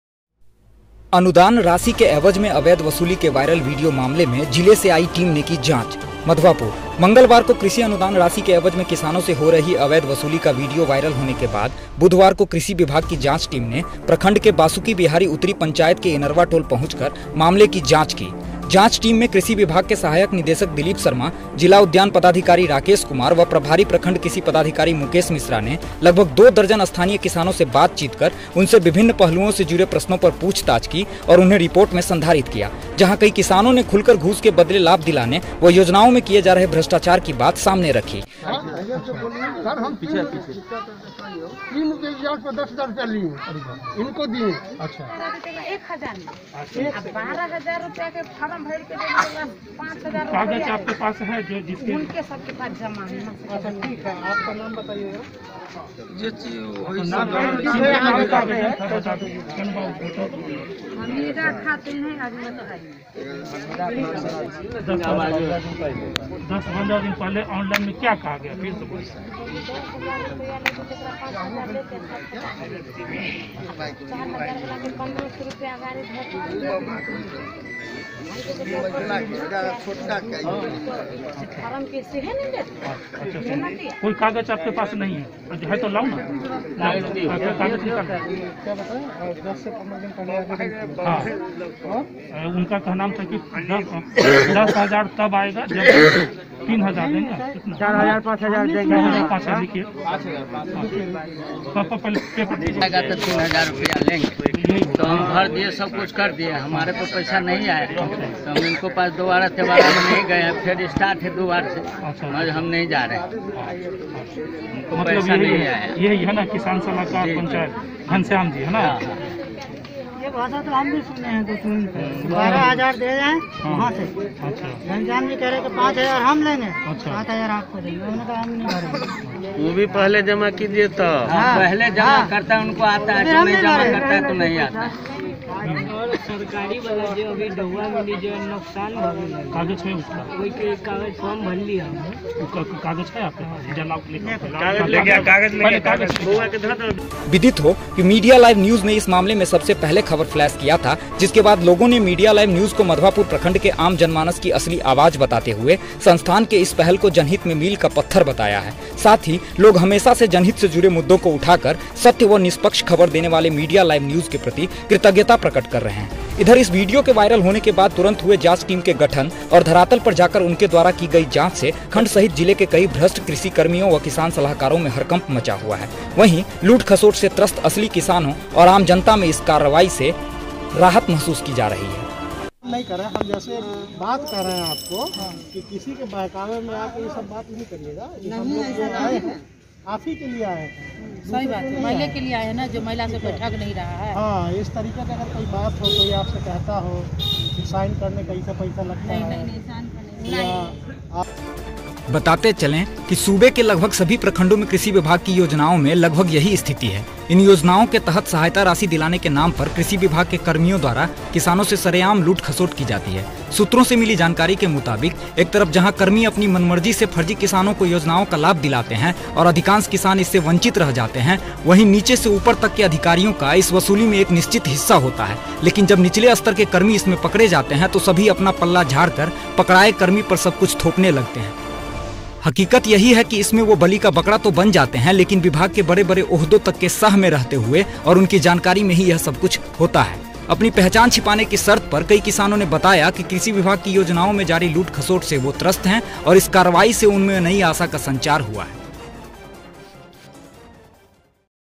medialivenews की खबर का असर : कृषिकर्मियों के वायरल वीडियो मामले में जांच को पहुंची जिलास्तरीय टीम
madhwapur प्रखंड के बासुकी बिहारी उतरी पंचायत से वायरल हुए वीडियो के मामले में जिलास्तरीय टीम ने धरातल पर पहुंचकर किसानों से की पूछताछ। रिपोर्ट अग्रसारित। publicnews publicproblems #madhubani